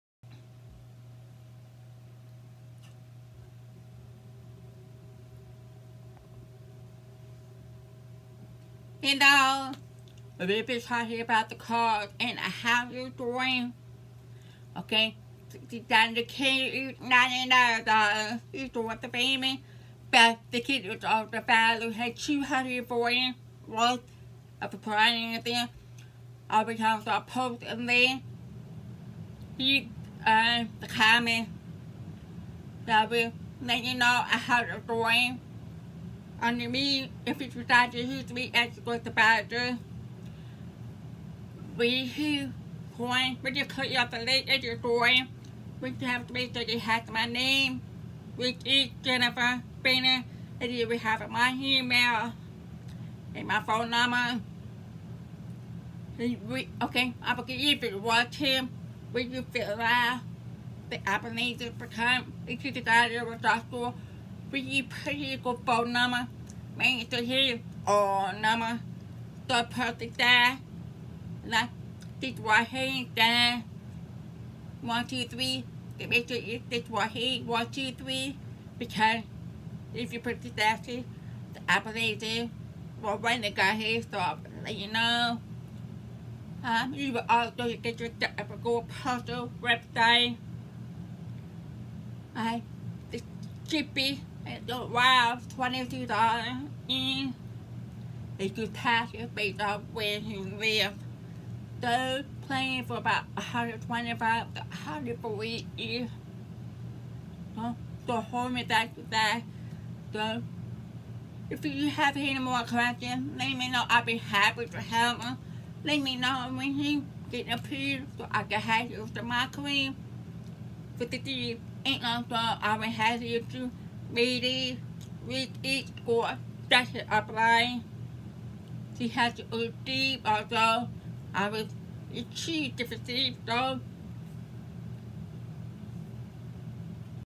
Cost & How to Join
Discussing about the cost and how to join Chalk Couture.